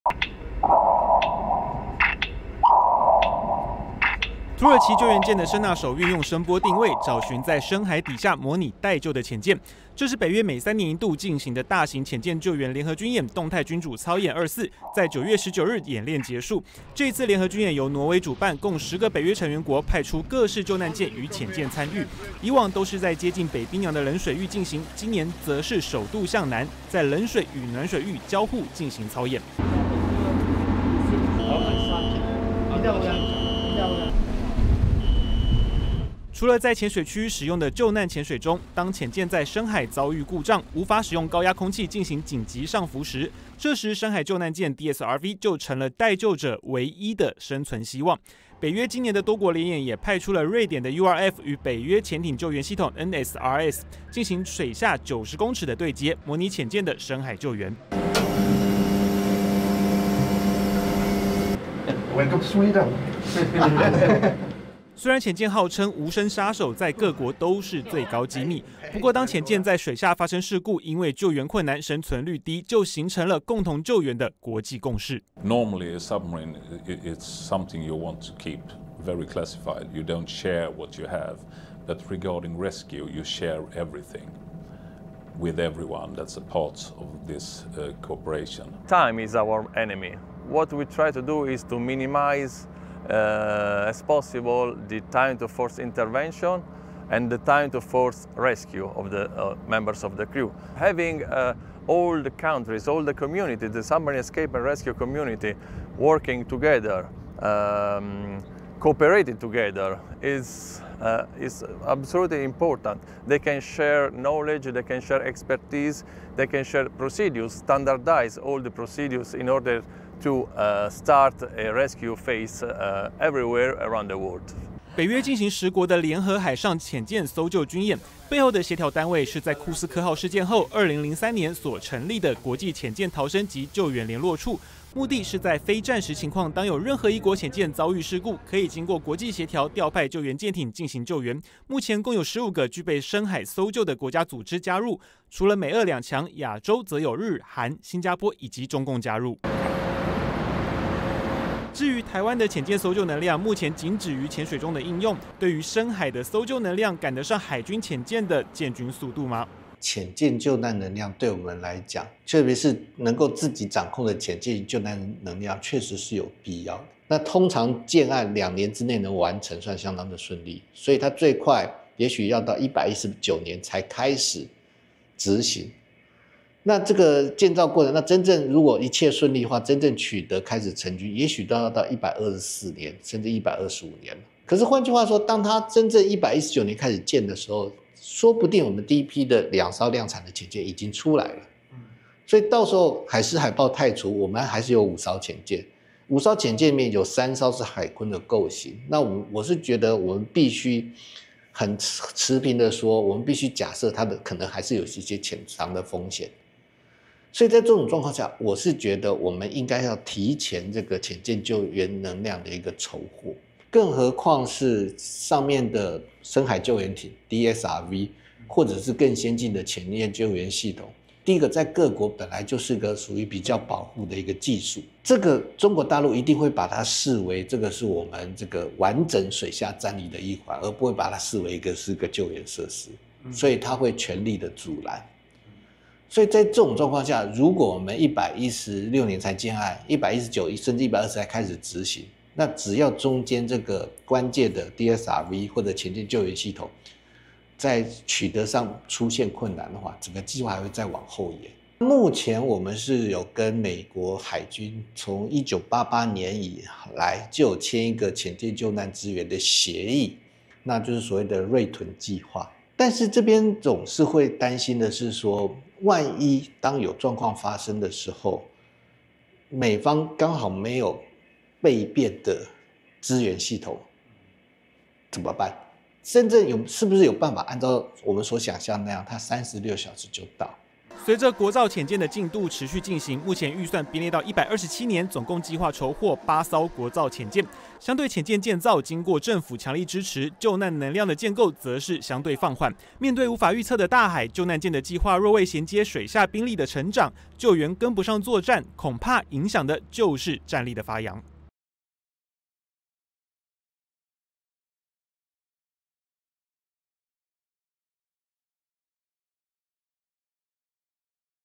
《軍情站》北約潛艦救援軍演十國參與 台潛艦救援單靠美軍藏孤立危機?
北約每3年一次的多國聯合潛艦救援軍演「動力君主24」(Dynamic Monarch24)，9月9日於挪威海域展開，並於今日結束。本次多國軍演共有包含美國在內共十個北約盟國艦艇參與，其中土耳其是首次以觀察員身分加入。

在聯合軍演中，除了演練運用救難船上的聲納系統尋找遇難潛艦，盟國之間也運用不同的深海救援潛艇(DSRV)進行操演，強化各盟國之間在潛艦救難上的互操作性。

由於潛艦救援相比生產潛艦的難度與成本更高，因此潛艦救援的能量相比潛艦的戰力更具有稀缺性，也使得許多國家建置潛艦救援的能量，多採用國際合作的模式進行。北約也因此成立了強調不限於北約成員國參與的國際潛艦逃生和救援聯絡辦公室(ISMERLO)，當世界各國的潛艦遇難時，只要是非交戰狀態下，都可經由組織的協調共同進行潛艦的救援行動。目前共有15個具備潛艦救援能力的成員國，除了美、俄兩強，亞洲國家亦包含日、韓、新加坡，與大陸解放軍。

海軍潛艦國造案(IDS)目前正如火如荼的進行，原型艦海鯤號目前正進行泊港測試，預計明年3至4月可進入海試，如果順利將可在明年11月交艦海軍。至於潛艦深海救難的能量，海軍參謀長邱俊榮表示，救難艦預計在116年進行建案，另與盟友也簽訂相互救援機制。目前海軍擁有2艘荷製的海龍級潛艦與2艘服役逾70年的茄比級潛艦，但並未建置深海救援能力。在未來海鯤艦與後續艦的加入後，潛艦的救援能量勢必成為迫切需求。中華戰略前瞻研究協會揭仲表示應將潛艦救難艦建案的期程提早，雖然與美方自1988年起簽訂了潛艦救援的「瑞豚」計畫，但美方的全球部署活動，是否在台灣有難時能有裝備即時支援，仍有不確定性。此外周遭國最近的日本，縱有深海救援能力，但我方與日本的軍事合作，日方態度都較為保守。而海鯤級潛艦尤其首批艦因是我國首次打造作戰潛艦，仍必須持平認為有相當的風險存在，在潛艦救難艦的籌獲上，也要考量水下救援載具的採購變數，如果遭受中共阻撓，勢必將再拖延建案期程。